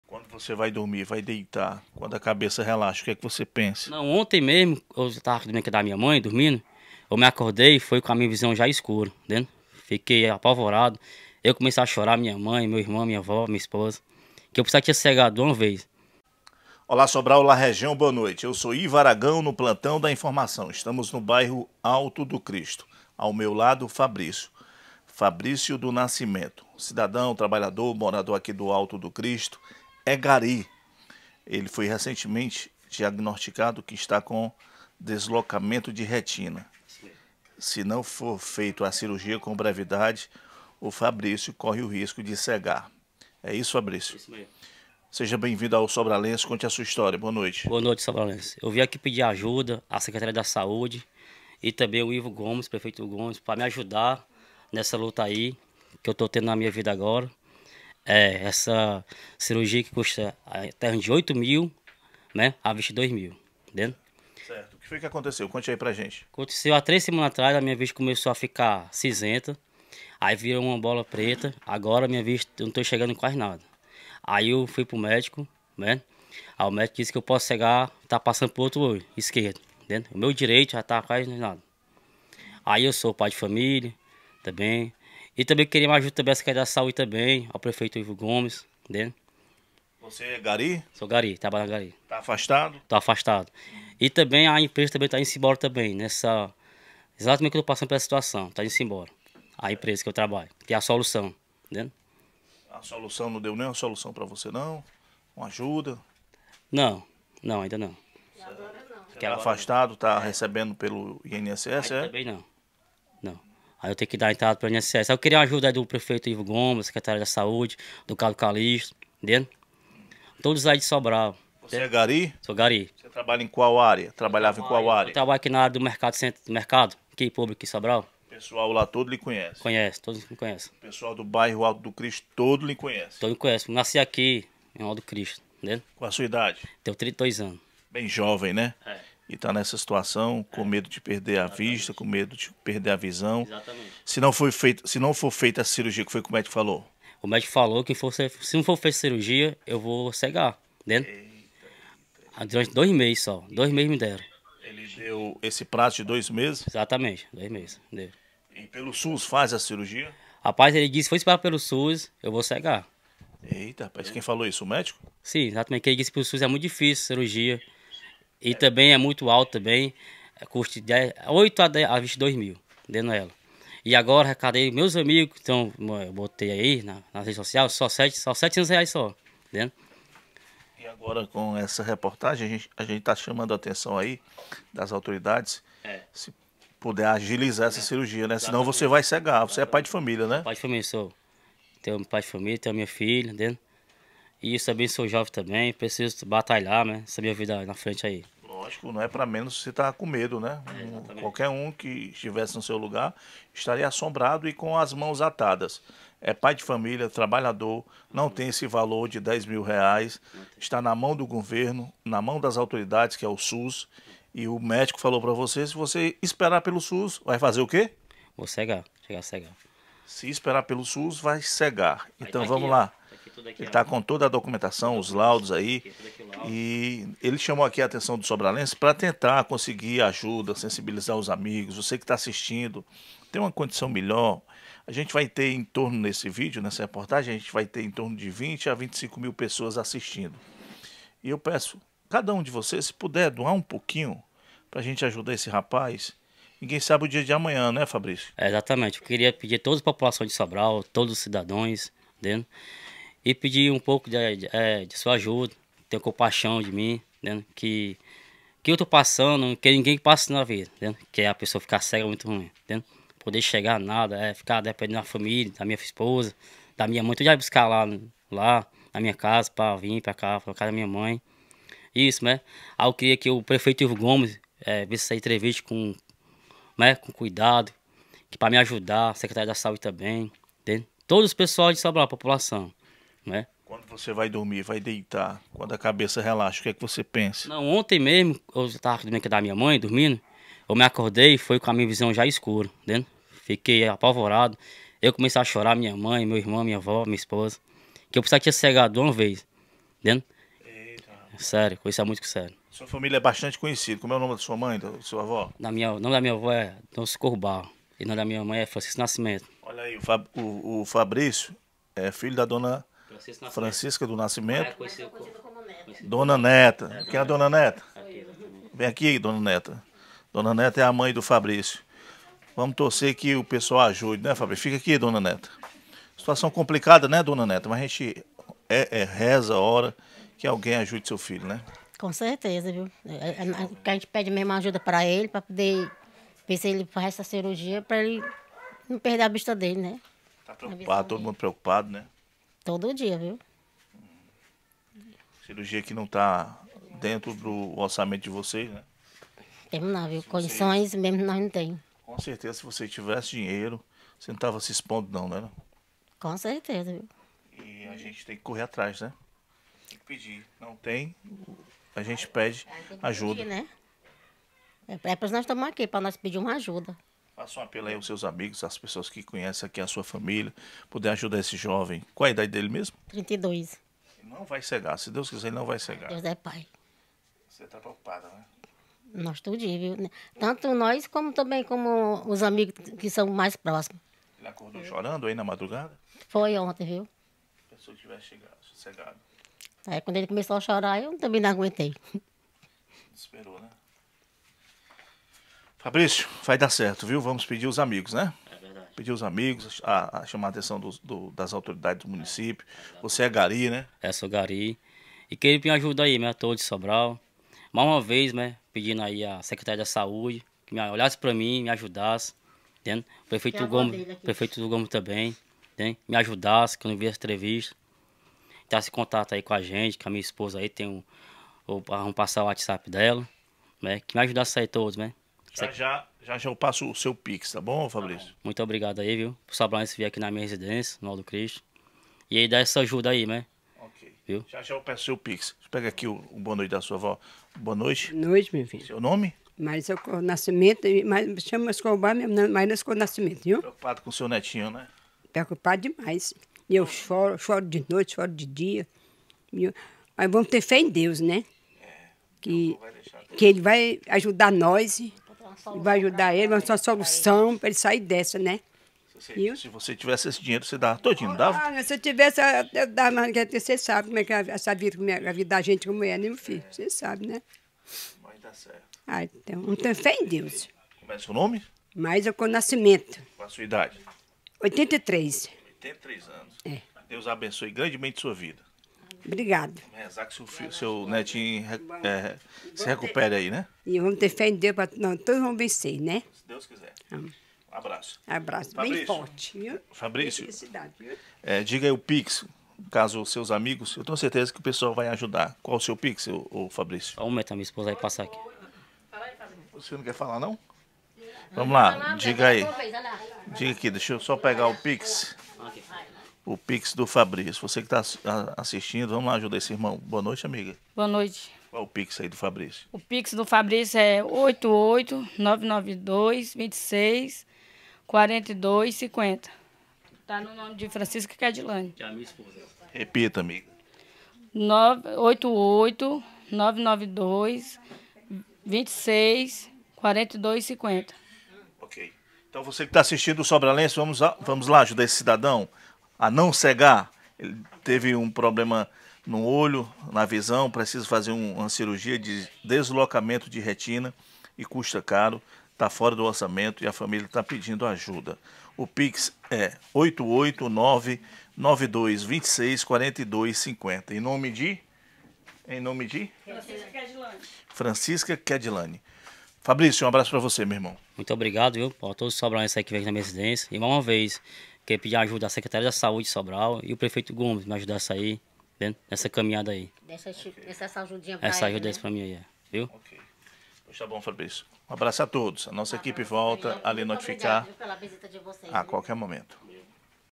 Se não fizer ele cega!!
Francisco Fabricio, trabalha como gari, é morador do bairro alto do Cristo, necessita fazer uma cirurgia para corrigir um deslocamento de retina que custa quase 10 mil reais. Ele resolveu apelar para rede social para tentar conseguir esse valor, se não ele vai cegar; o médico falou que ele tem dois meses para realizar a cirurgia. Pix: (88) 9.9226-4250 - Francisca Kedilane
Eu sou Ivo Aragão, no plantão da informação!!

''Eu sou a ressurreição e a vida. Quem crê em mim, ainda que morra, viverá; e quem vive e crê em mim nunca morrerá'', Jesus Cristo.